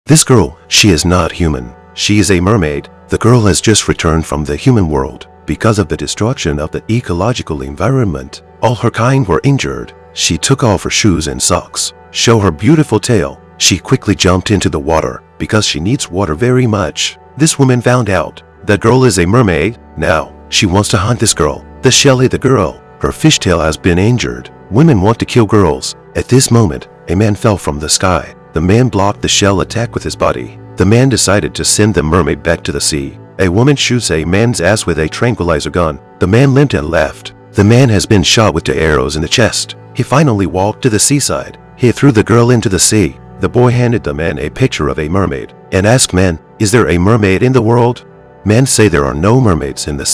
The girl is a mermaid.She is being hunted.@Movie Life
The girl is a mermaid.She is being hunted.A man descends from the sky, stabbed in the chest to protect her.!@Movie Life